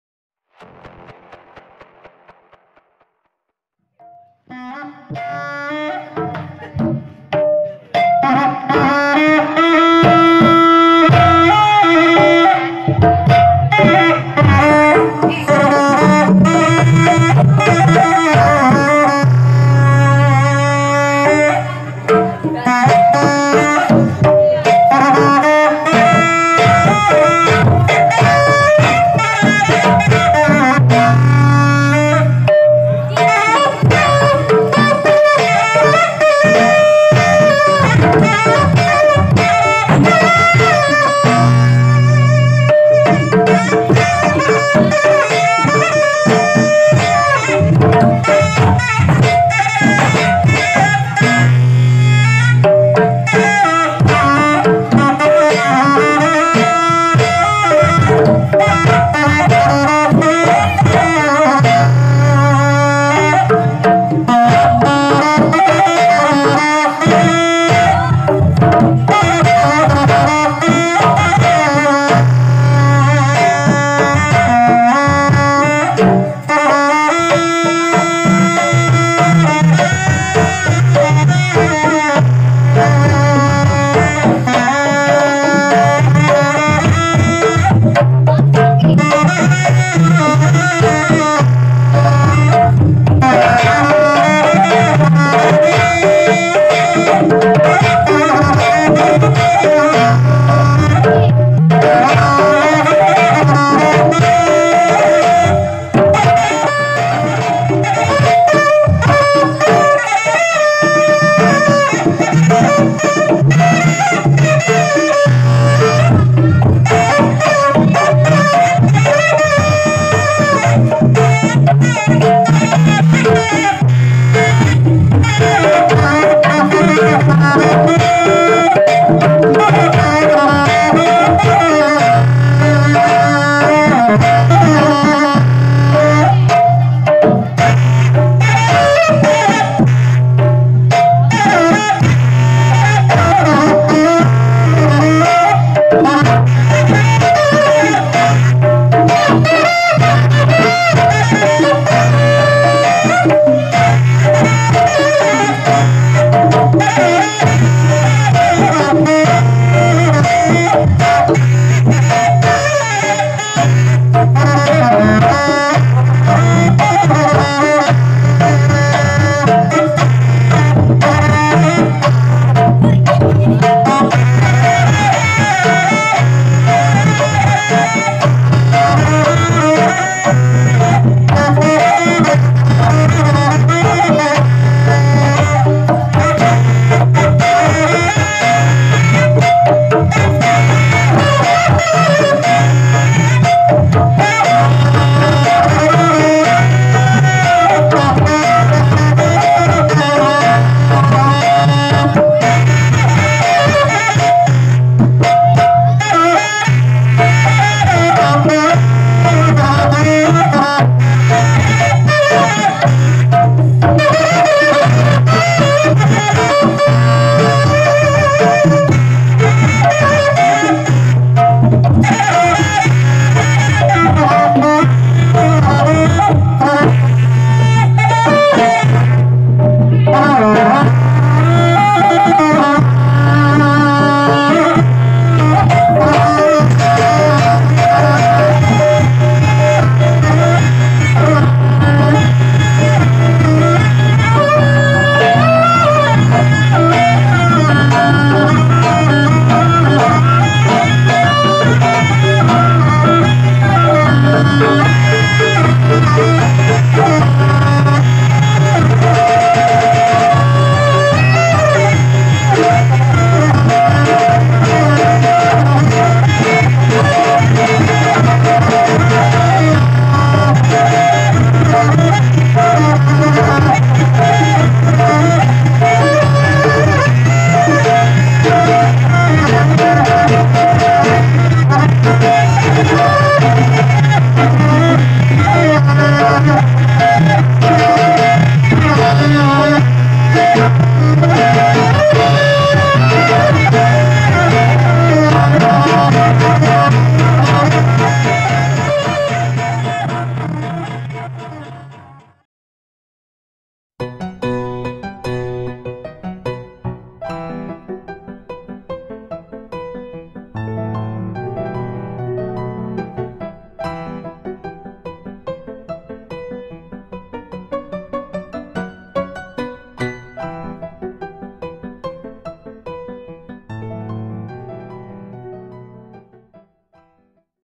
Ibing Tunggal Tepak 2 || Ibing Tepak 2 Naek Tepak 3 || Sinar Pusaka Langeng Kamulyan
Oke kali ini kami mengupload video ibing (Kembang) silat dalam peragaan tepak 2 naek tepak 3
video kali ini diambil pada era new normal dengan mematuhi protokol kesehatan sesuai anjuran pemerintah, jadi tetap safety yaa🙏